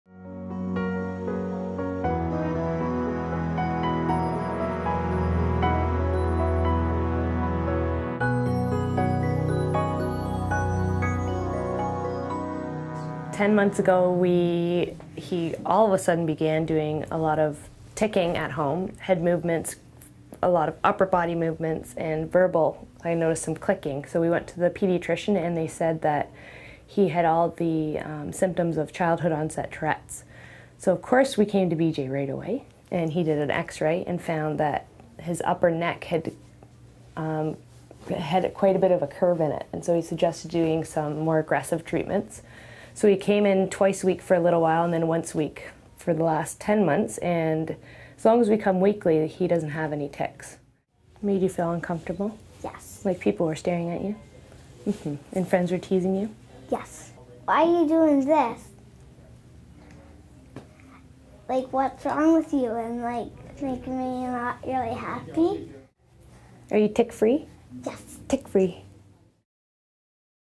Tourettes & Chiropractic
Kaila and Jase share how specific chiropractic adjustments resolved symptoms of childhood-onset tourette's syndrome. "As long as we come weekly, he doesn't have any ticks."